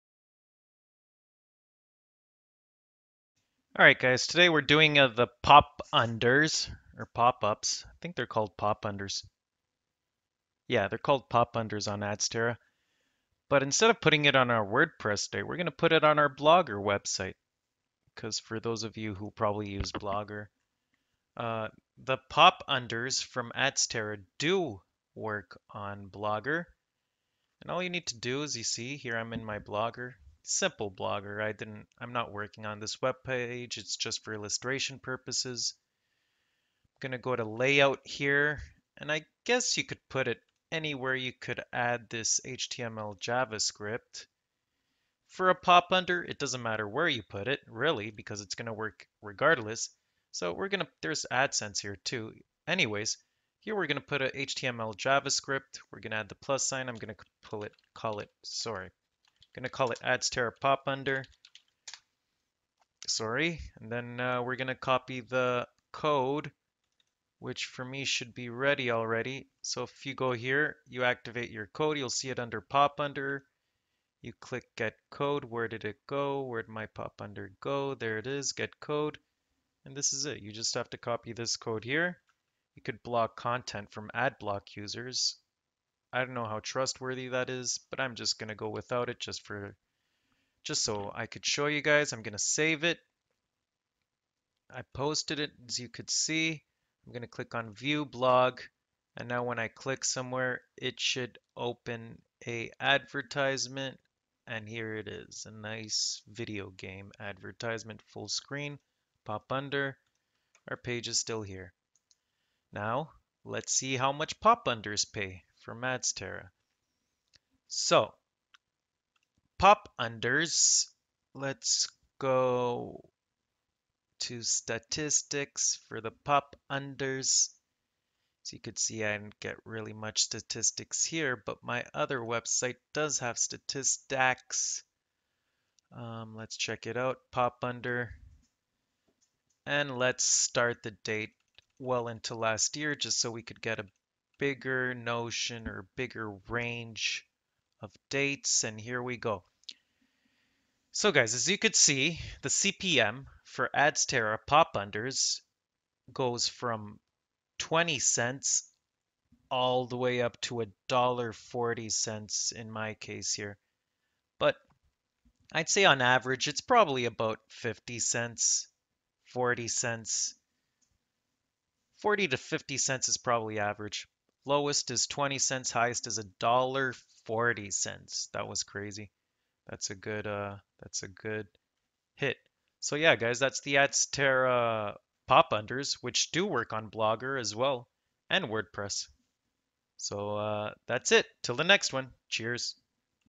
ADSTERRA POP UNDER ADS,CPM in BLOGGER SITE. How much do they make?
In this video I add a adsterra pop-under to my blogger site, Test it out, and find out how much CPM I earn from it.